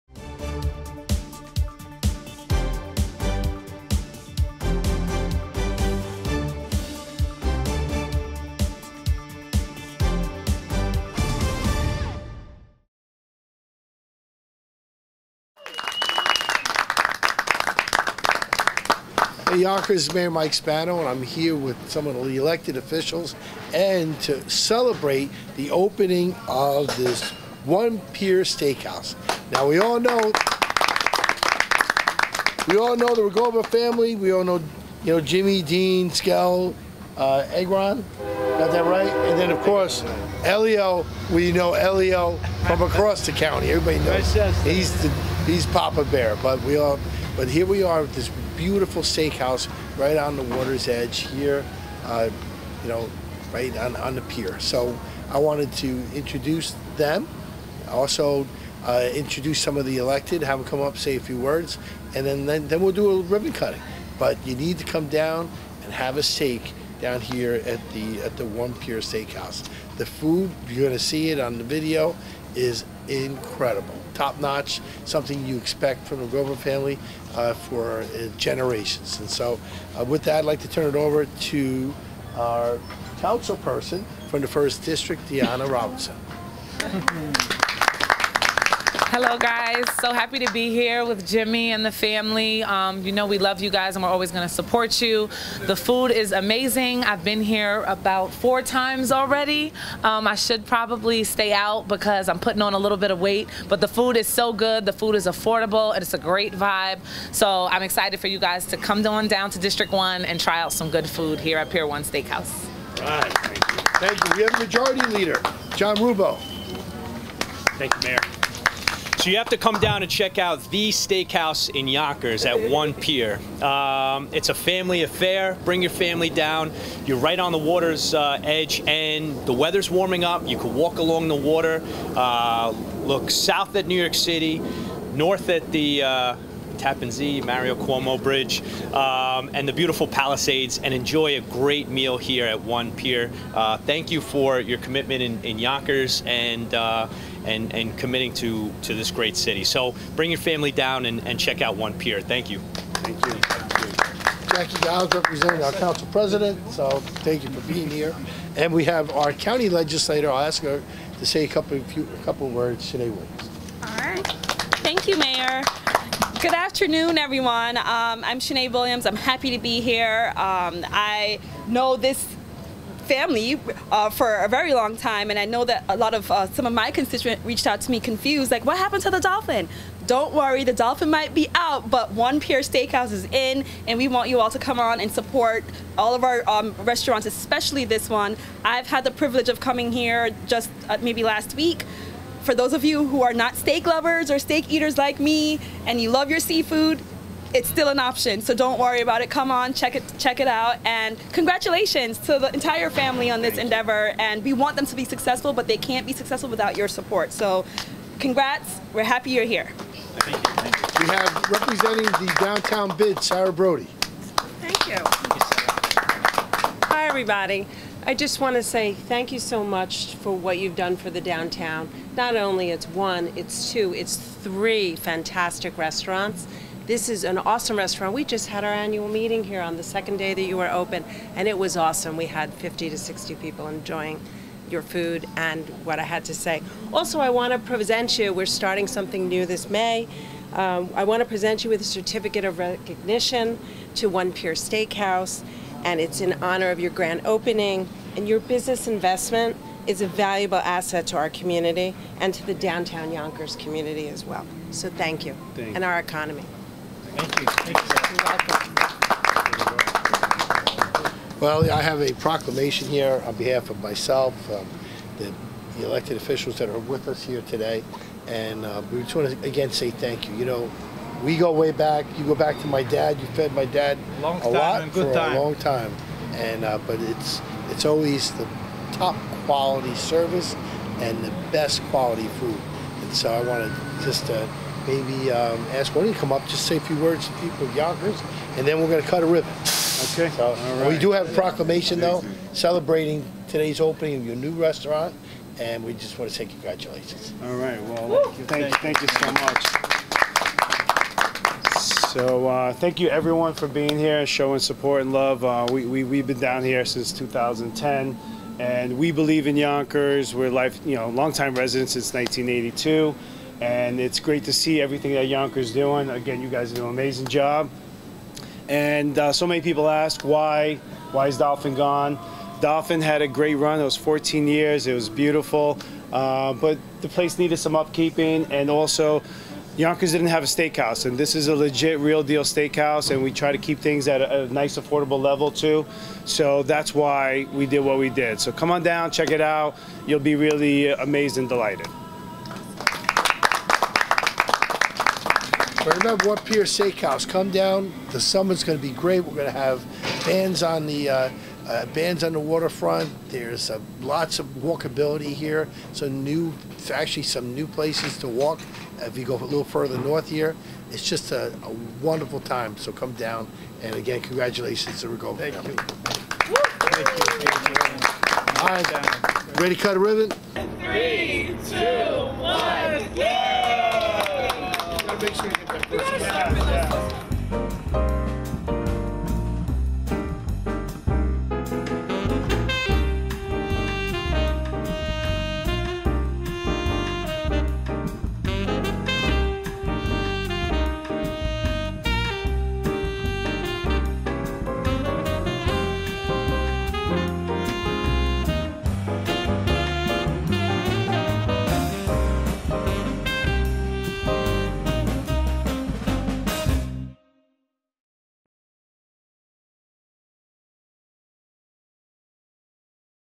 Ribbon Cutting: One Pier Steakhouse
Excited to welcome One Pier Steakhouse to Downtown Yonkers!

Taking over from Dolphin Restaurant, indulge in quality steaks, fresh-off-the-boat seafood, and a variety of drinks. Stop by 1 Van Der Donck St!